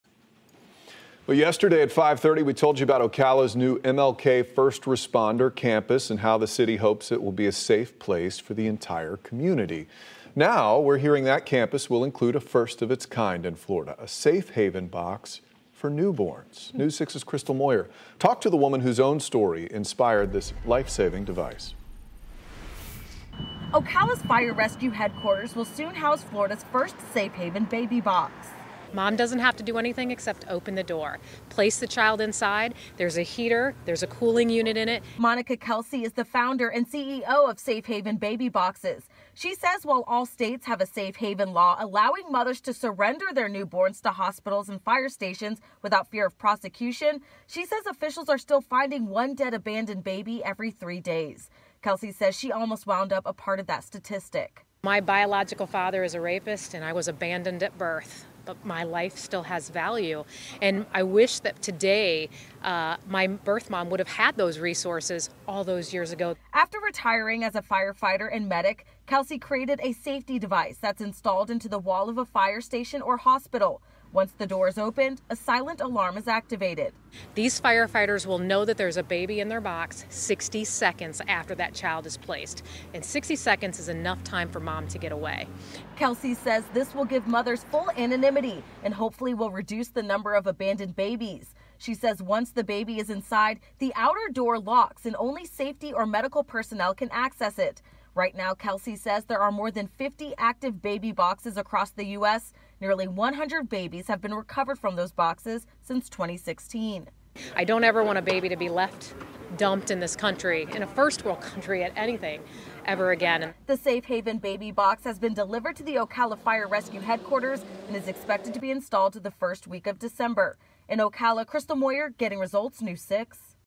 Ocala to install Florida’s first Safe Haven Baby Box
Ocala’s Fire Rescue Headquarters will soon house Florida’s first Safe Haven Baby Box.